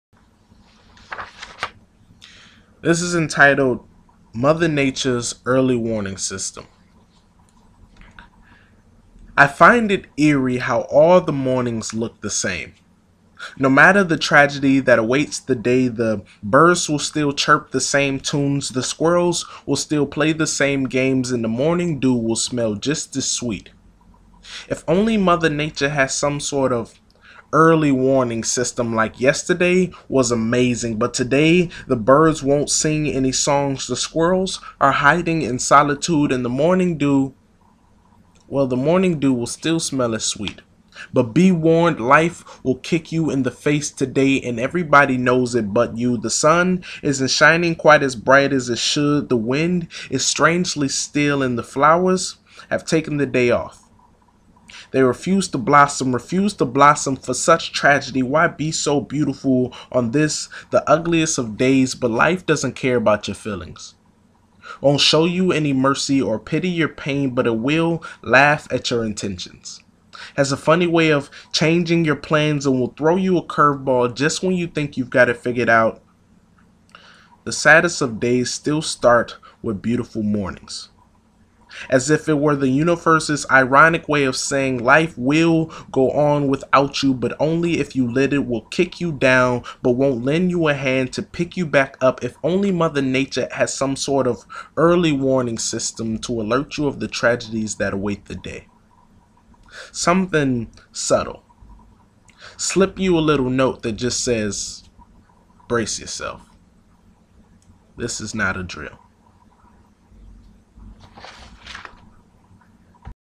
Poem: Mother Nature's Early Warning System
written and performed by Eric iRhyme Spann. Read from "Restless Days & Sleepless Nights"